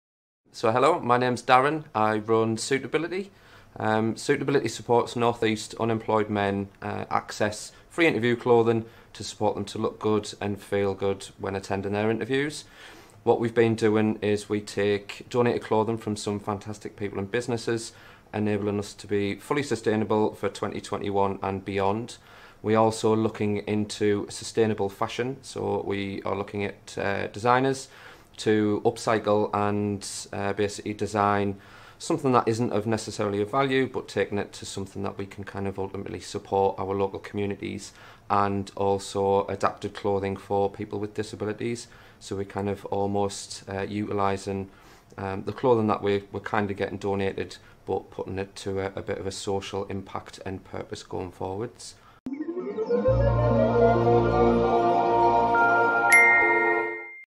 Darren Tate, founder of Suitability - Pitch
Suitability founder Darren Tate's pitch from Startup Awards North East 2021